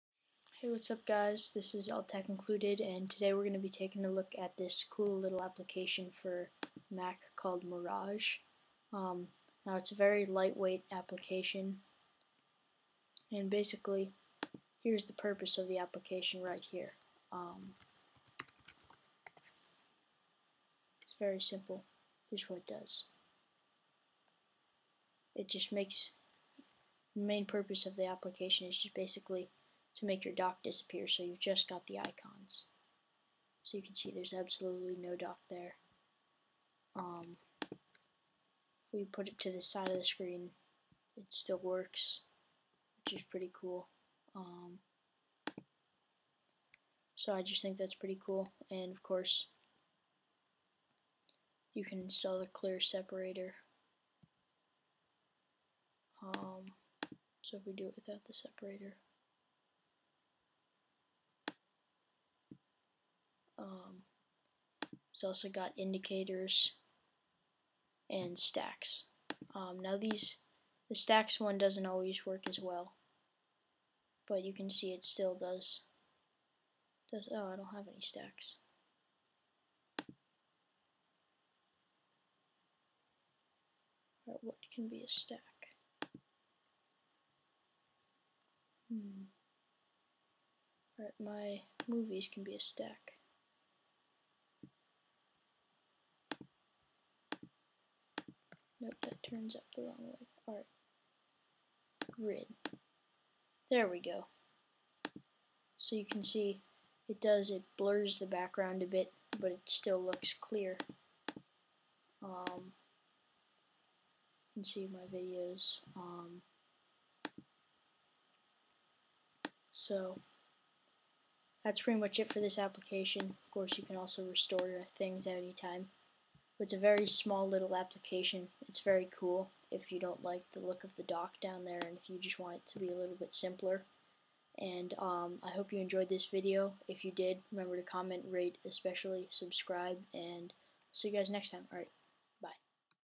Mirage for Mac Review- Make your dock disappear!
A review/tour of Mirage for mac, a free application that can make the background of your dock disappear, leaving just the icons.